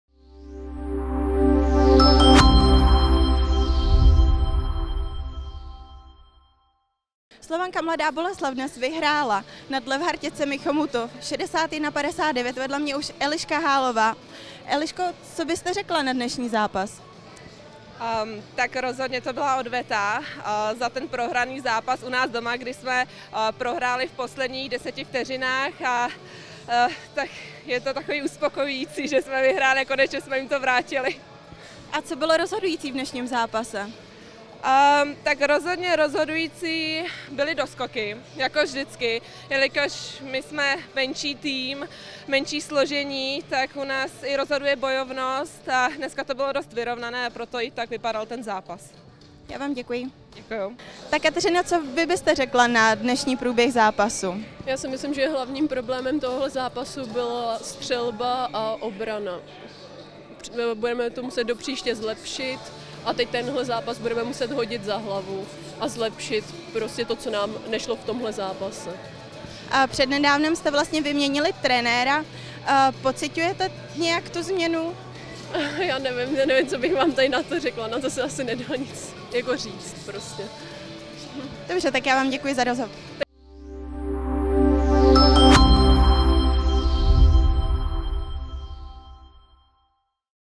BK Levharti Chomutov vs. Slovanka MB - 6/3/2020
Pozápasový rozhovor s Eliškou Hálovou (Slovanka MB) a Kateřinou Rokošovou (BK Levhati Chomutov) . Konečný stav zápasu - 61:59 pro Slovanku.